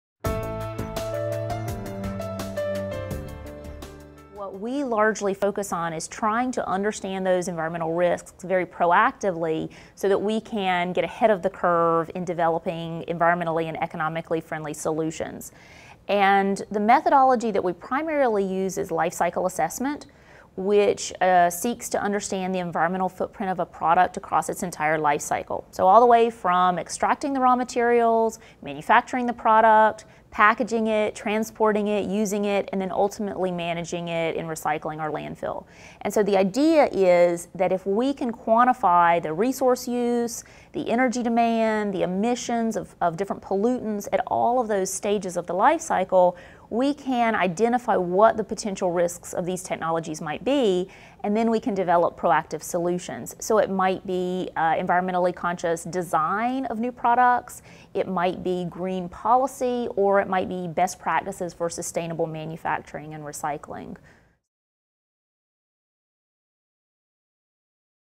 RIT Expert: Callie Babbitt, associate professor, Golisano Institute for Sustainability
Callie Babbitt, associate professor in the Golisano Institute for Sustainability at Rochester Institute of Technology, is an expert in sustainability, sustainability research and education, and sustainable waste management.